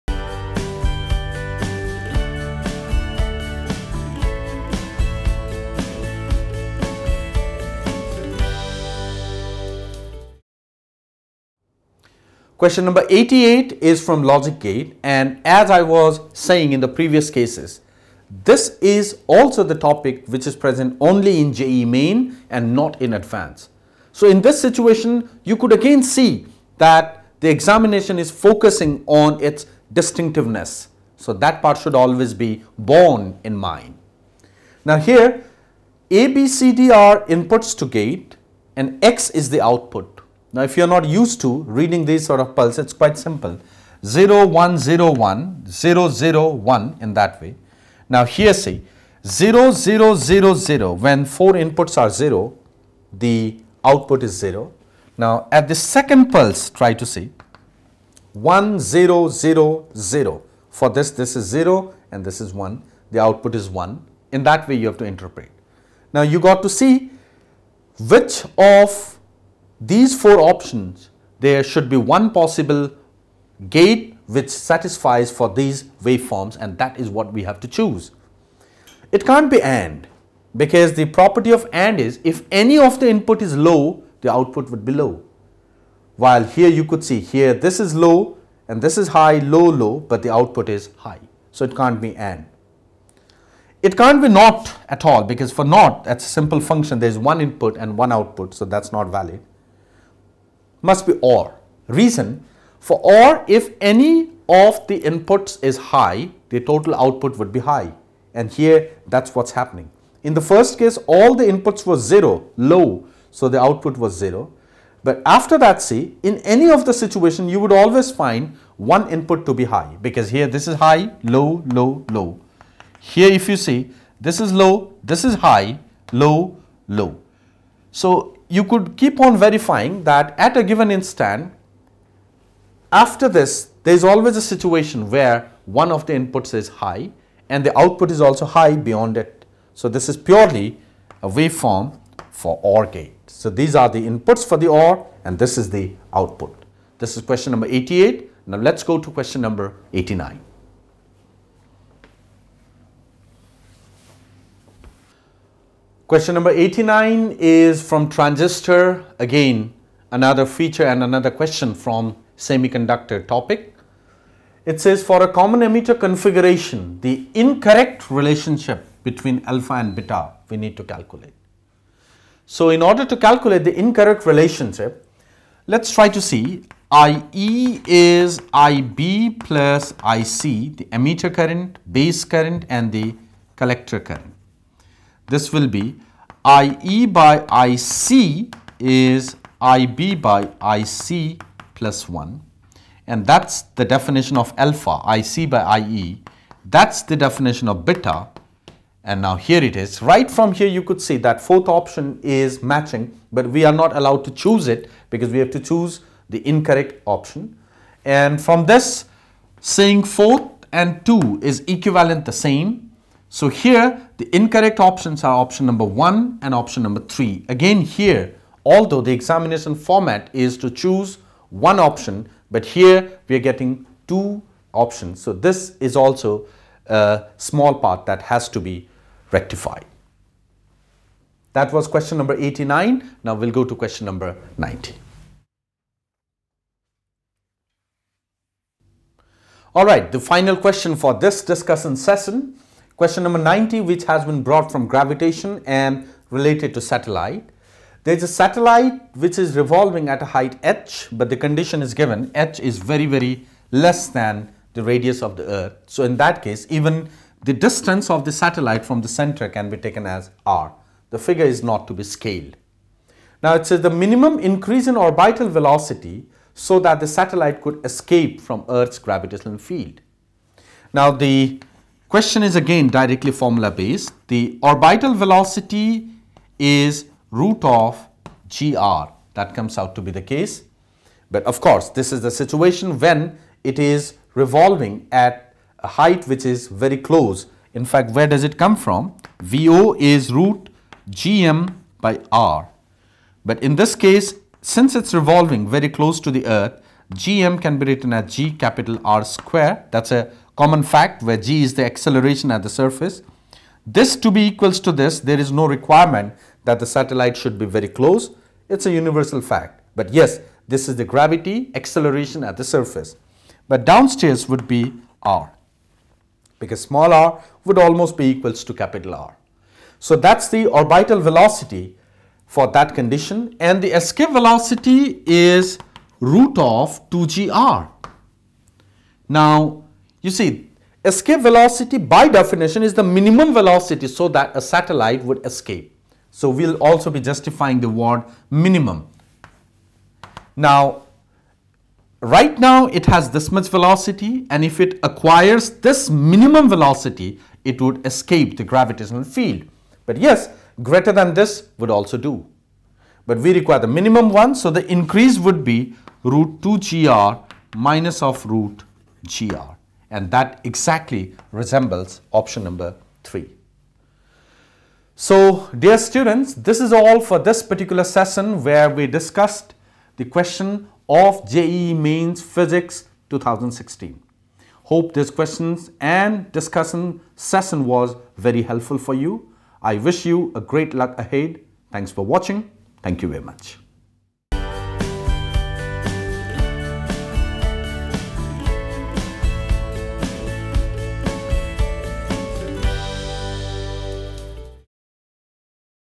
JEE-Main 2016 Physics Solution [Q. 88-90] By Aakash Faculty
Find JEE (Main) 2016 Video Solutions of Physics Paper from Question No. 88-to-90 (Code-F) by Aakash. [Apply here for JEE (Advanced) Crash Course